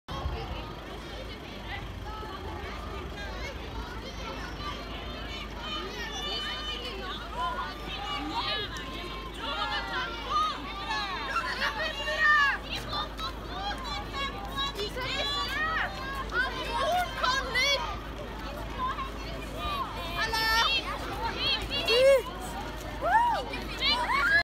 Greverud skole øver på 17. mai-tog
Det er dagen før dagen, og sang og trinn skal sitte.